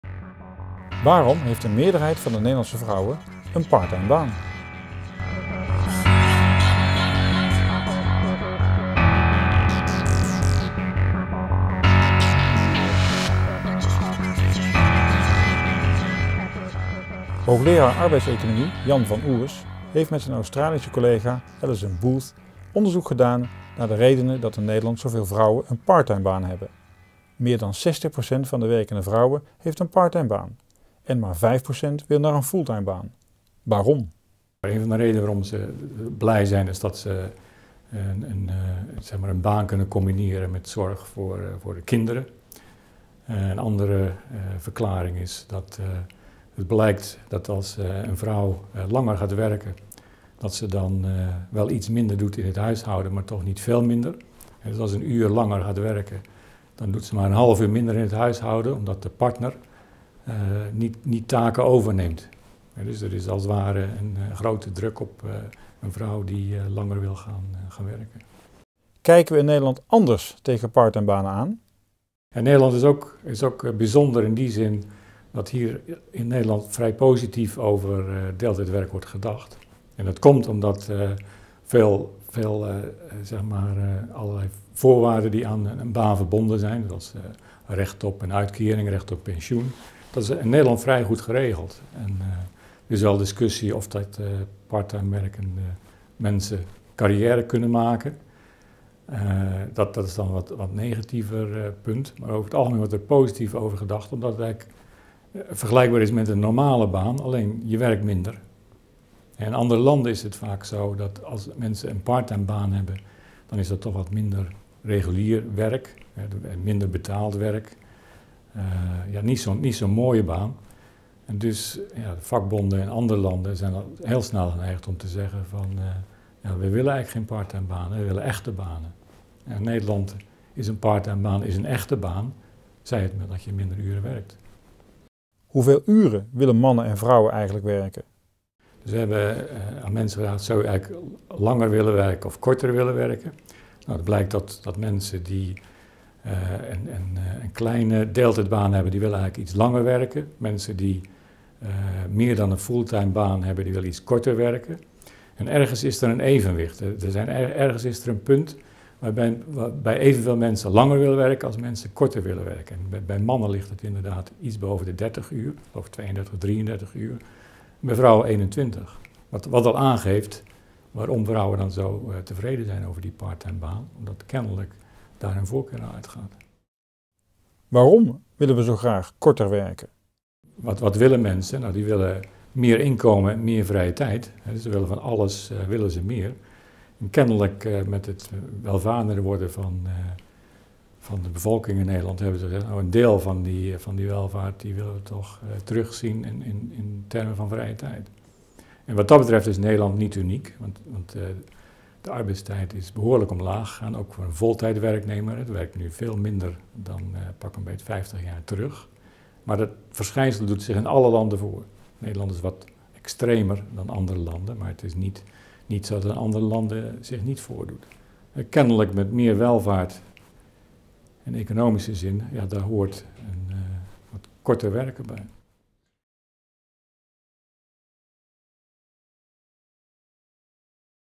Econtrack - Jan van Ours over parttime werken van vrouwen
De meeste Nederlandse vrouwen met een partner zijn gelukkig met hun deeltijdbaan. Dat blijkt uit onderzoek van de Tilburgse hoogleraar Jan van Ours en zijn Australische collega Allison Booth. Deeltijdwerken is voor hen geen tussenfase op weg naar een fulltime baan, maar het einddoel. Het is wat de Nederlandse vrouw wil.